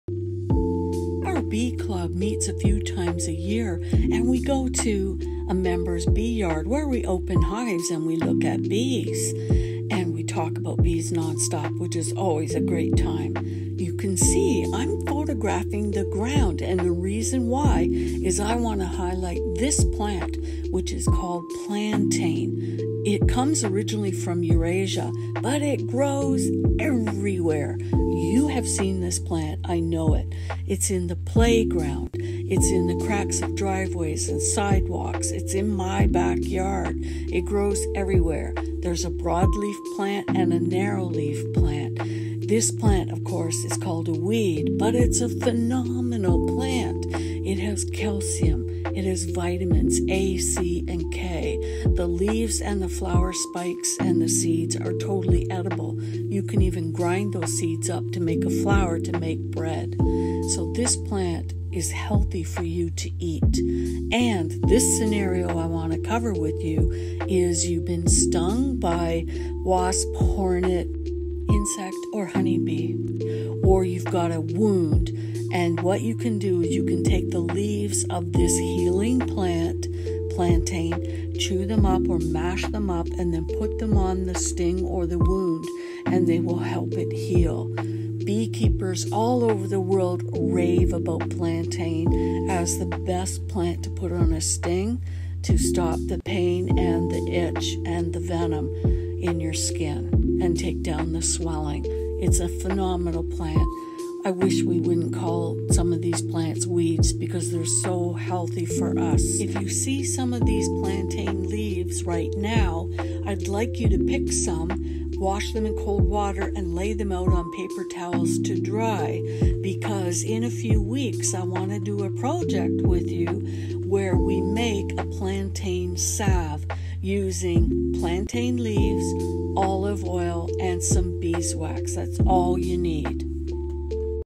Bee sting, insect sting, or wound care in the wilderness with Plantain
Plantain is the weed that originated from Eurasia but you can currently find it absolutely everywhere it’s in the school yards. It’s in backyards it’s mixed in with grass grows in the cracks of concrete and tarmac. It’s a very common weed but this weed is special it’s completely edible, and the mashed up leaves of this plant are great to apply to an insect bite, a bee sting, or a wound, because they promote healing many beekeepers like to make a salve using plantain